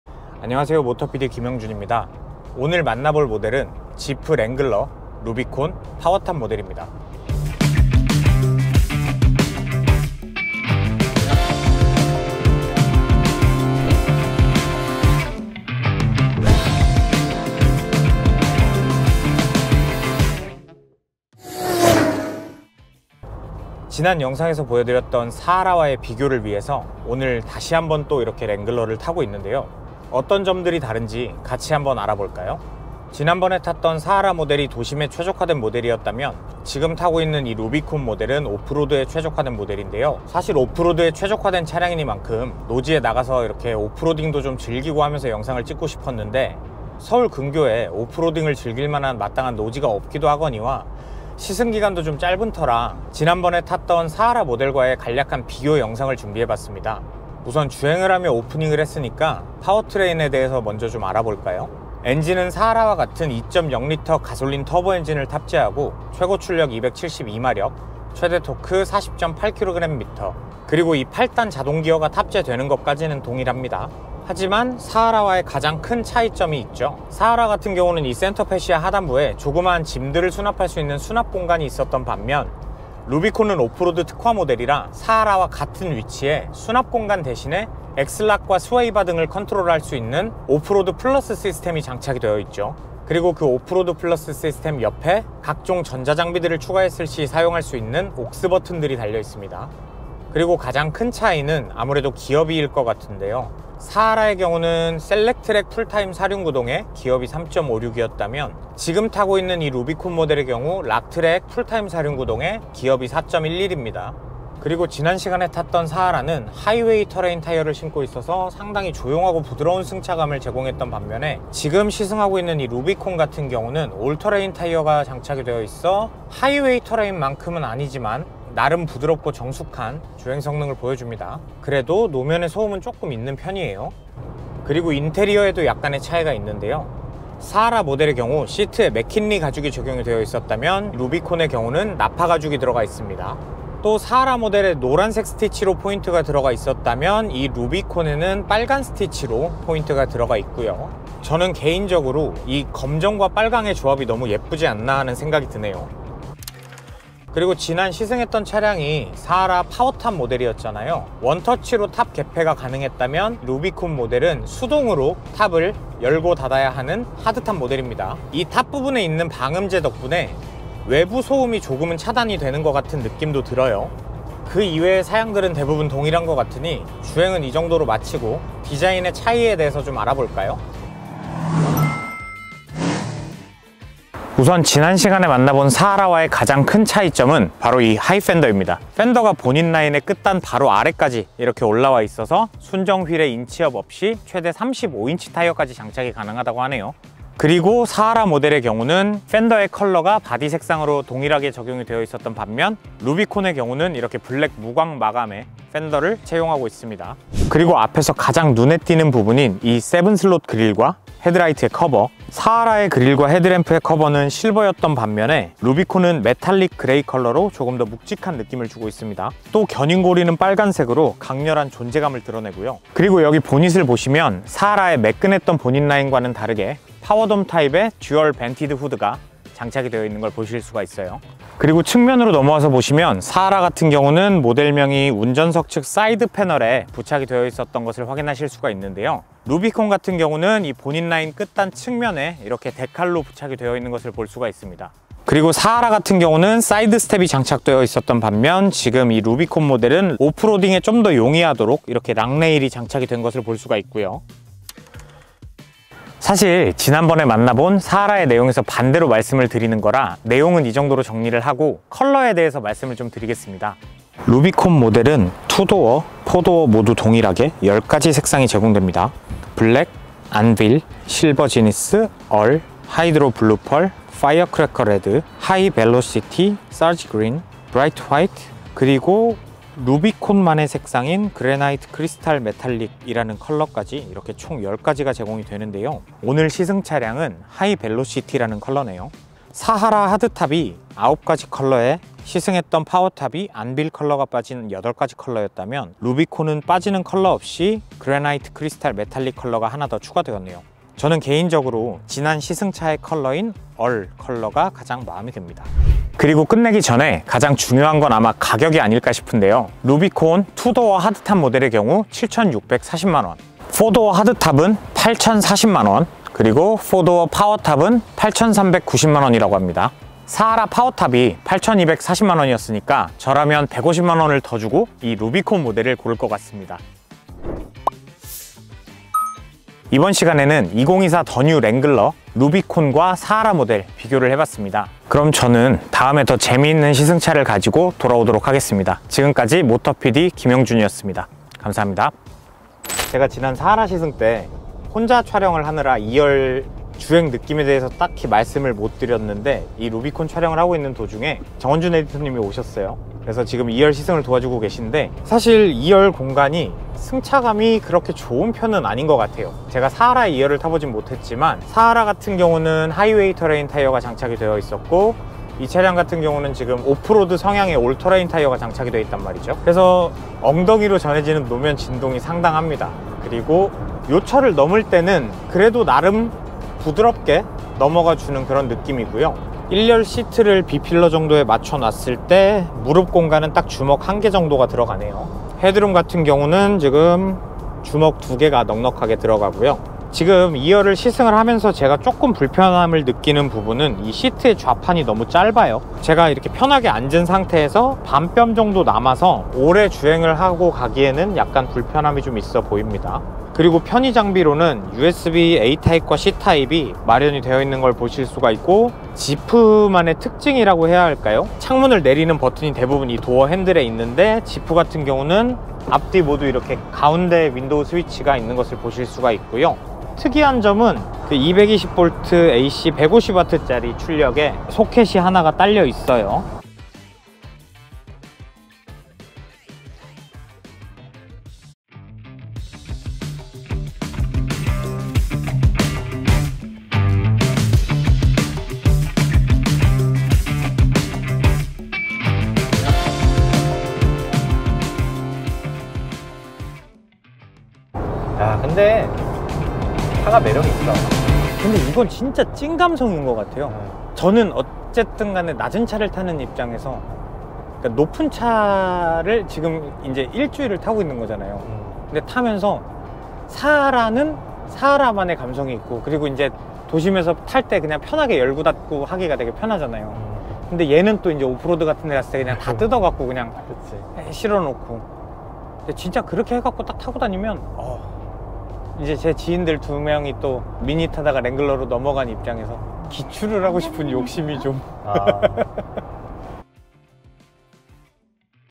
지프 더 뉴 2024 랭글러 루비콘 하드톱, 사하라와 비교하면?
‘랭글러’는 지프 브랜드의 살아있는 역사일 뿐만 아니라 SUV의 기준을 정립해 온 상징적인 모델입니다. 전설적인 4x4 주행 성능으로 오프로드 마니아들에게 오랜 기간 인기를 얻으며 ‘지상 최강의 SUV’로서 명성을 이어가고 있습니다. 2017년 글로벌 공개 후 6년 만에 부분변경을 거친 ‘더 뉴 랭글러’는 새로워진 디자인과 강화된 편의 안전 품목으로 현대적인 변화를 꾀했습니다. 지프는 온·오프로드를 넘나드는 주행 성능과 랭글러 특유의 감성으로 일상과 일탈을 자유롭게 넘나드는 라이프스타일을 적극 공략할 방침이라고 전했습니다.